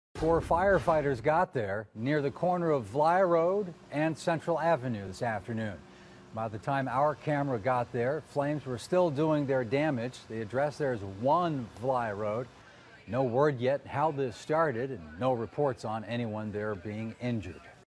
Fire on Vly road in Coloine
COLOINE - Fire crews battled a blaze on Vly road.

Multiple fire departments responded after fire broke out near the corner of Vly road and Central Avenue at around 3:30 on Sunday.

Crews fought the blaze for several hours before extinguishing the fire.

The cause is under investigation.

(photo: Trevor English)